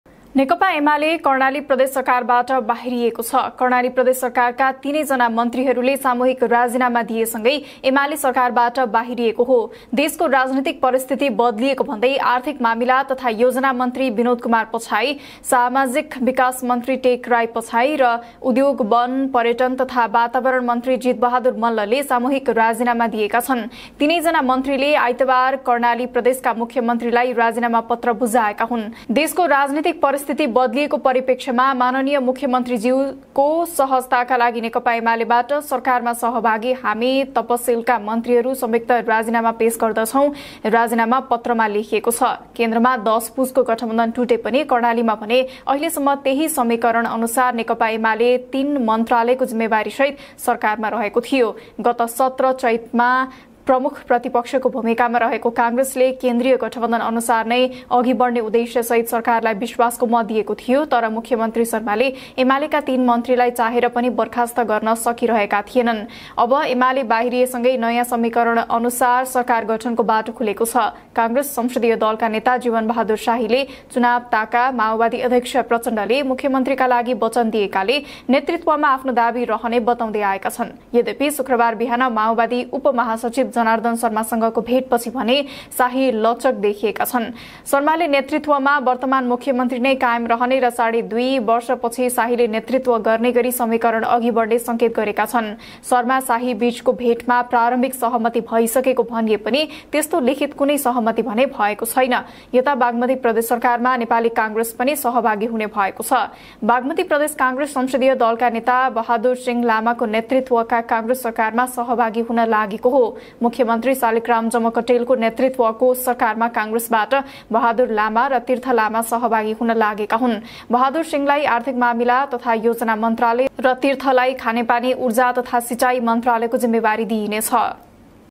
प्रदेशमा नयाँ खेल,कर्णालीबाट एमाले आउट|| Nepal Times
Produced By: Nepal Times Media Private Limited
News Edited By: Harihar Nepal
Presented By: Rashmita Udash
Video Edited By : Nepal Times